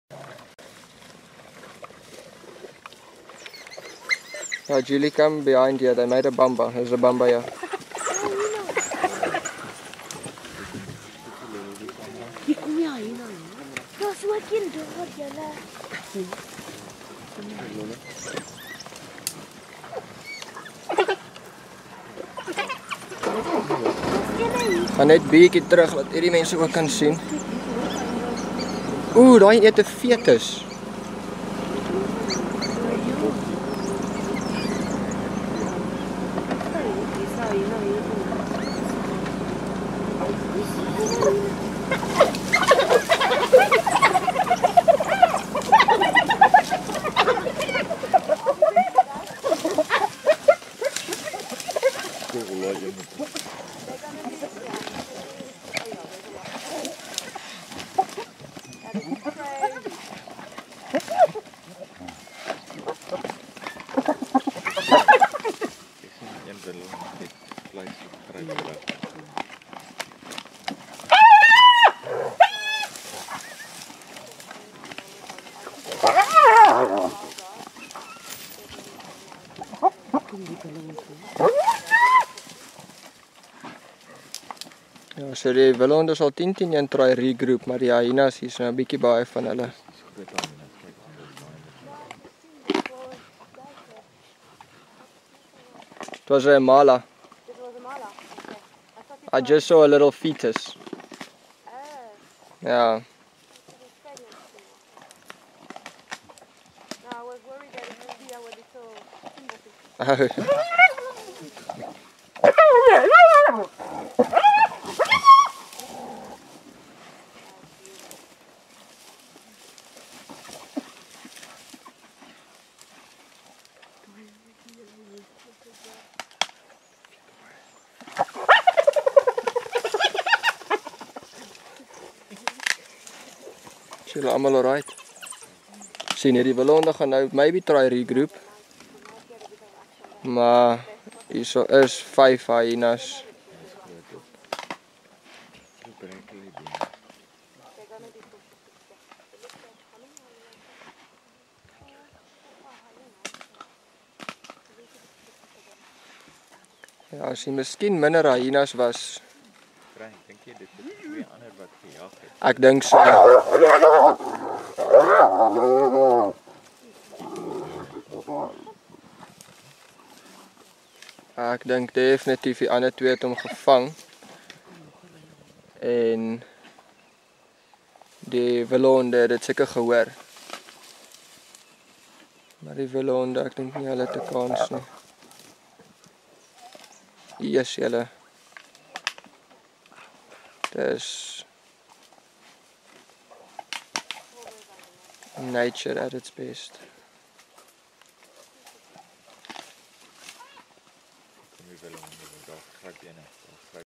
Wild dogs vs hyena
We spend some quility time with a pack of African Wild dogs at a waterhole when suddenly they all got up and ran in the the same direction. I had a very good feeling there might be a kill nearby as 2 dogs dissappear a little time earlier. We raced around the corner we came across 2 dogs that took just took down a female Impala and was already eating. The rest joined the short lived feast. One got hold of a fetus and can be seen in the clip. A couple of minutes past and a gang of hyenas came storming in  stealing the hard earned meal.